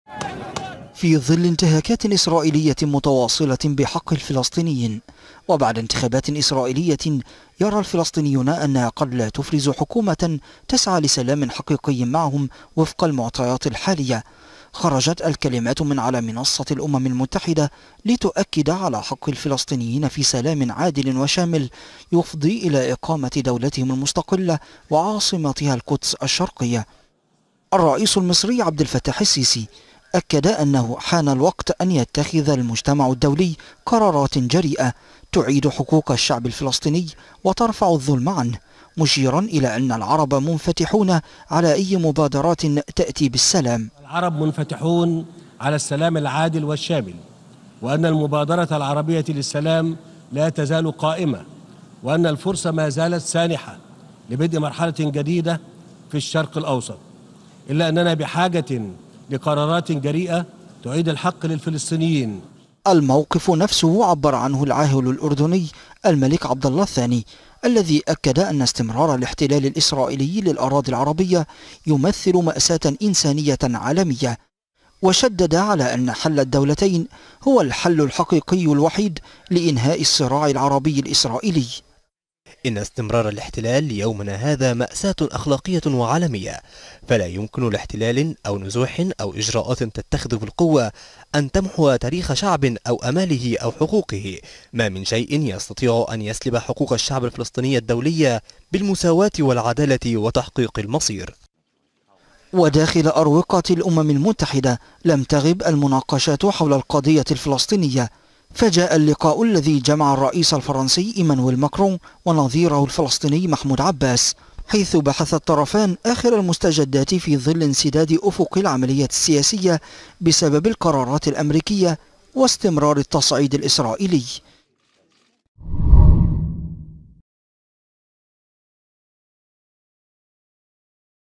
القادة العرب في الأمم المتحدة يؤكدون حق الفلسطينيين في إقامة دولة مستقلة
أكد القادة العرب في كلماتهم أمام الجمعية العامة للأمم_المتحدة على مركزية القضية_الفلسطينية ، إذ حذر العاهل_الاردني الملك عبد الله الثاني ان العالم كله سيدفع ثمن استمرار الصراع العربي الإسرائيلي، وفي كلمته أمام اجتماعات الجمعية العامة للأمم المتحدة أكد العاهل الأردني أن حل الدولتين هو الحل الوحيد للقضية الفلسطينية ..من جانبه أكد الرئيس المصري عبد الفتاح السيسي على ضرورة حل القضية الفلسطينية وفق قرارات الشرعية الدولية ومبدأ حل الدولتين.. المزيد من التفاصيل عبر التقرير المصور.